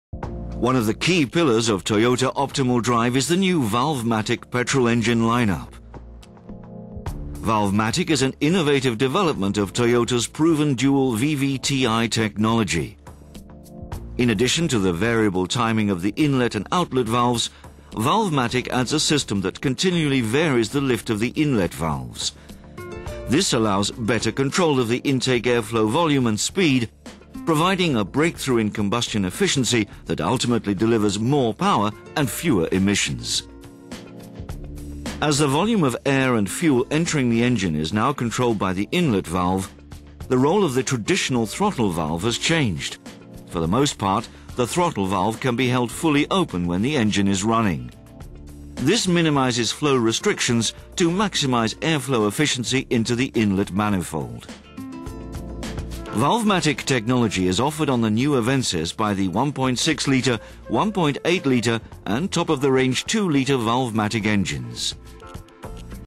Toyota Valvematic variabele klepbediening
de Avensis introduceerde Toyota benzinemotoren met Valvematic-bediening voor de inlaatkleppen. Deze aanvulling op VVTi-verstelling voor de nokkenassen maakt ook de lichthoogte van de kleppen variabel.
De video laat zien dat er een extra as bij komt kijken, waarop tuimelaars draaien die de inlaatkleppen bedienen. Deze tuimelaars zijn tweedelig, en via de extra hulpas verstelbaar. Hierbij verandert de stand van de tuimelaarvinger die op de nokkenas loopt, ten opzichte van de vingers die de kleppen indrukken.
Zo kan simpel gesteld de klepspeling groter of kleiner worden gemaakt. Hoe groter de speling, hoe minder de lichthoogte van de inlaatkleppen. Mooi aan dit mechaniek is dat het weinig extra ruimte inneemt, de motor wordt nauwelijks hoger of breder. Heel erg belangrijk, gezien de eisen aan voetgangersveiligheid, die behoorlijk wat vrije ruimte tussen motor en motorkap vergen.